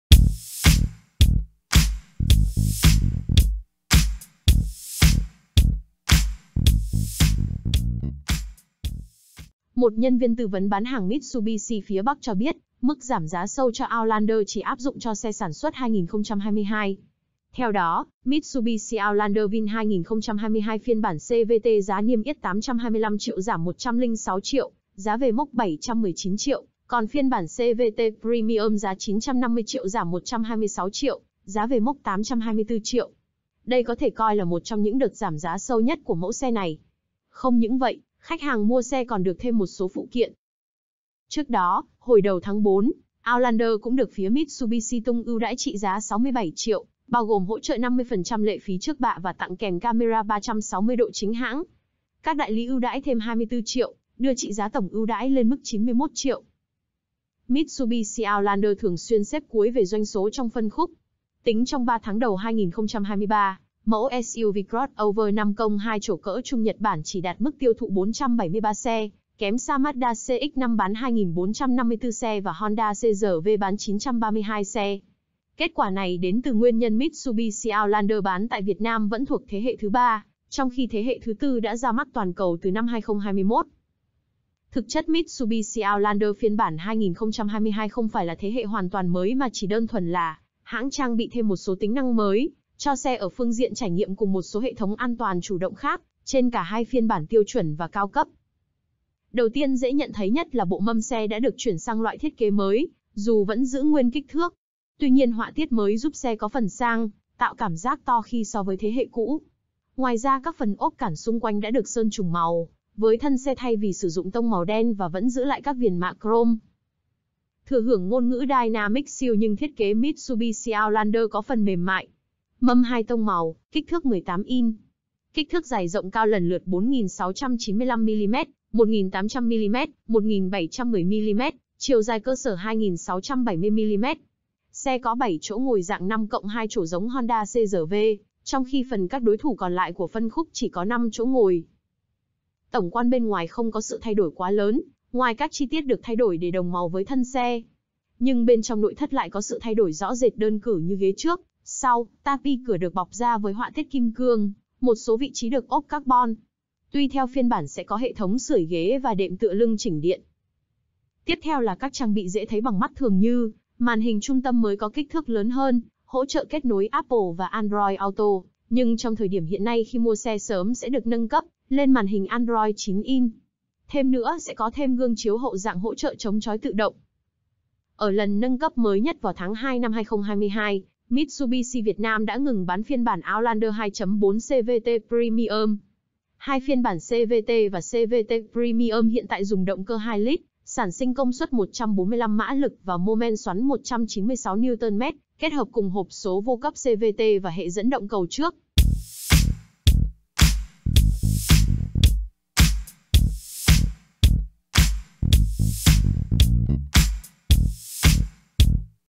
Giảm kịch sàn tại đại lý, SUV 5+2 chổ giá chỉ còn hơn 700 triệu đang khiến Honda CRV "sợ té khói"
Mitsubishi Outlander phiên bản CVT giảm 106 triệu, giá về mốc 719 triệu đồng, còn phiên bản CVT Premium giảm 126 triệu, giá về mốc 824 triệu đồng. Đây có thể coi là một trong những đợt giảm giá sâu nhất của mẫu xe này. Không những vậy, khách hàng mua xe còn được thêm một số phụ kiện.